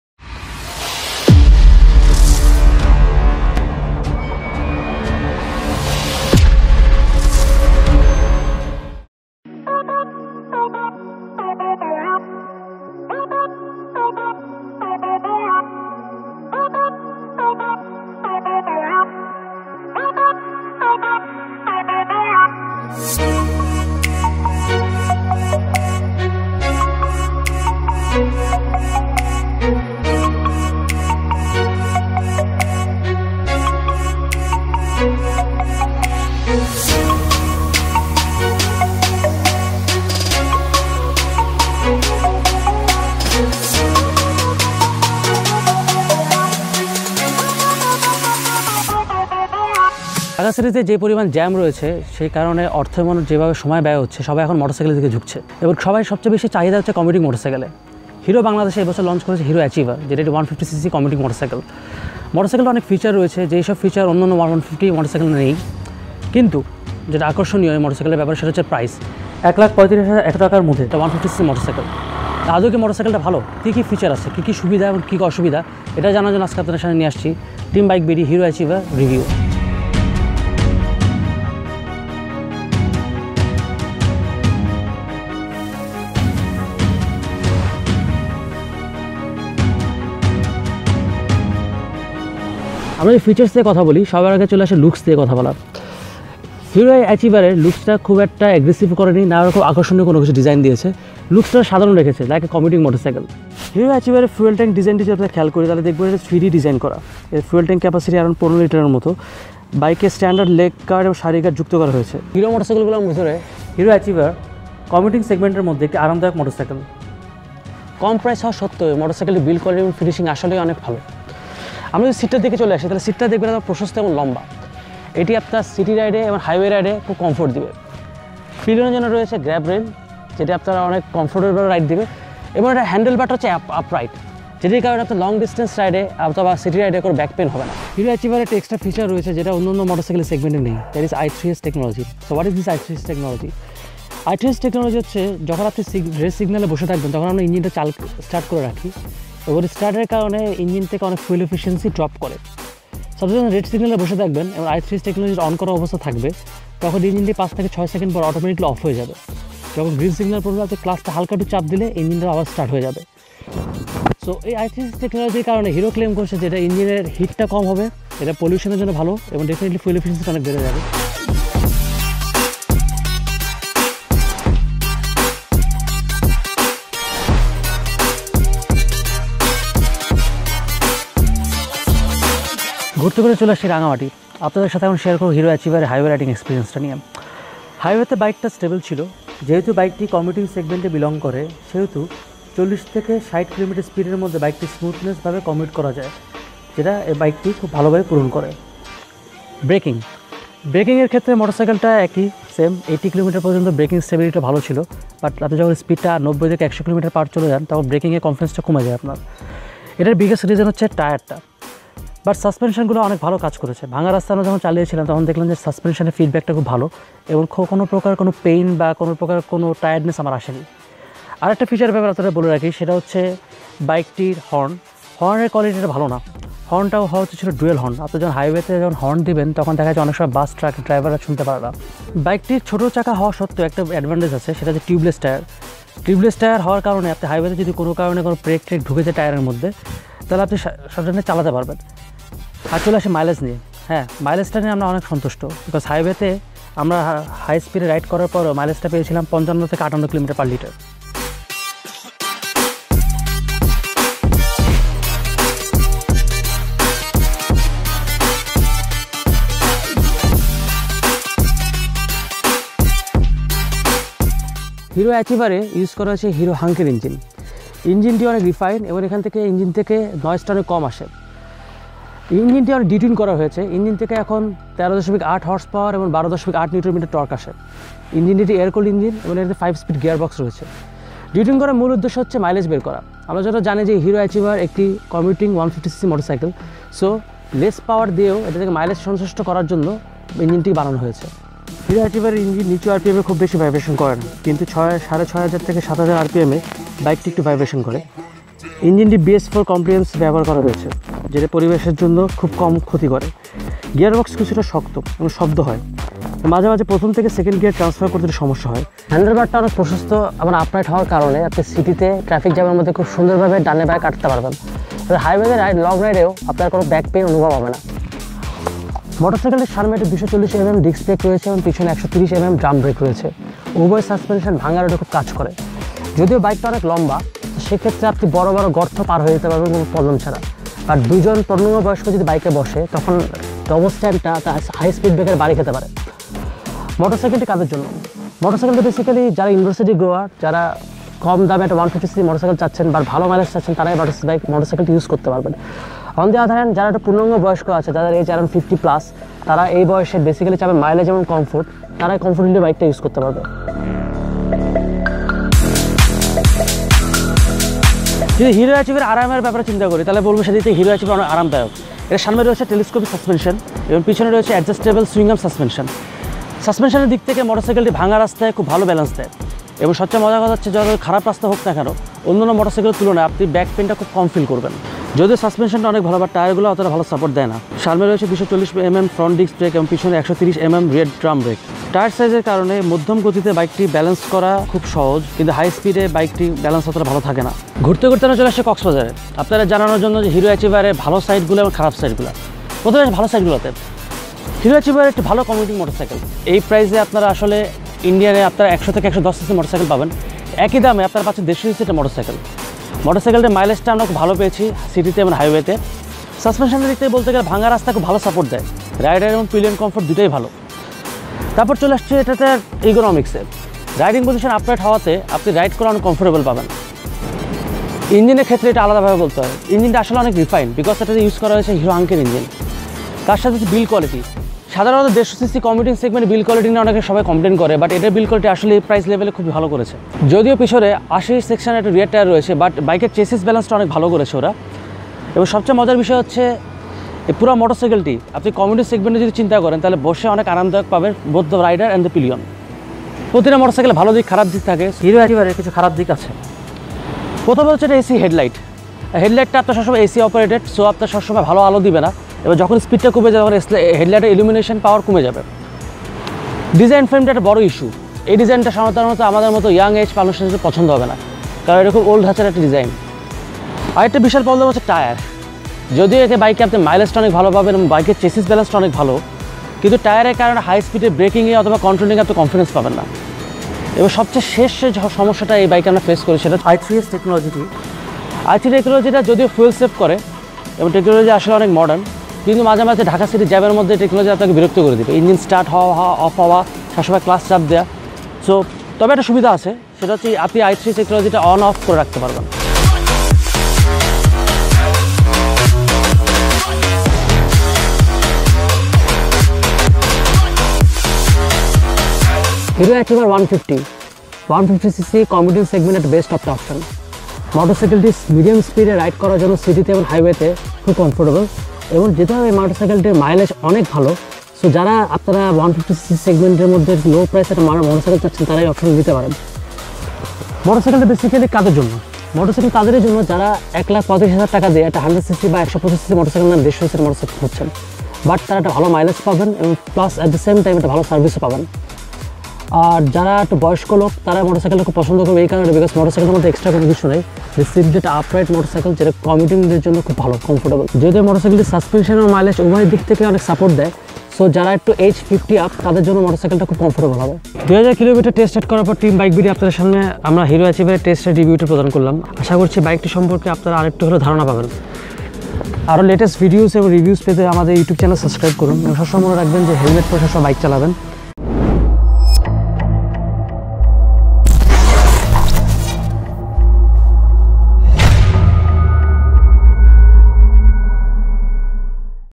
Hero Achiever 150 Review By Team BikeBD
Hero Achiever Test Ride Review Team BikeBD : Most Economical Indian 150cc Motorcycle in Bangladesh

Day by day the demand for commuting motorcycles is increasing in Bangladesh. With hectic traffic jams and to save both money & time for many people motorcycles now became a part of need in life like mobile phone. Hero Motocorp is one of those companies who always concentrated in the commuting segments and to add to that they introduced Hero Achiever in Bangladesh. After extensively testing for 3000 km both in city & on the highway we present you the Hero Achiever Test Ride Review Team BikeBD: Most economical Indian 150cc motorcycle in Bangladesh.

Hero Achiever Test Ride Overview

Hero Achiever was launched in Bangladesh on June 15th of 2017. The main purpose of this motorcycle from Hero was to give the Bangladeshi bikers the taste of 150cc motorcycle with the price of a 100-110 cc motorcycle. Currently the bike is priced at 135,100 BDT. Hero Achiever is for those people who want mileage & comfort in this segment.

To start with the bike we have to start with the looks, it isn’t aggressive but rather they kept the looks normal. They didn’t add any tank scoop or any sort of extra cosmetic makeup to the bike, they kept the look as simple as possible. The headlight of the bike is fitted with AHO (Automatic Headlight On) which means the headlight will be turn on whenever the engine is turned on but it won’t put any pressure on the battery.

They did add a 3D design muscular fuel tank which adds more themes to the bike. Fuel tank can carry up to 13 liters (Reserve 1.8 Ltrs) of fuel. They gave the bike standard leg guard which as we tested is very strong. Indicators are bulb type & the tail light design is something not very attractive with us.

In terms of build quality & finishing they did a proper job with Hero Achiever. Given the price of the bike it is very hard to find this type of finishing in a 150cc motorcycle. I think overall it will get good marks in the build quality department. The red color metal fuel tank does glaze when it sits under the sun.

Hero Achiever Test Ride Packed With Commuting Features

Hero Achiever is the tight pack of commuting features. Here the seat is very wide & long and also comfortable to sit on. It has an upright riding position & the pillion sit has rear grab rail for holding.  It has standard saree guard and extra foot peg to help keep both the legs of women on the same side. There is a long exhaust pipe with metal plate on it to keep the heat away from the foot of the pillion. The chain cover is fully covered which will help to keep out the dust & mud away and also will help its longevity.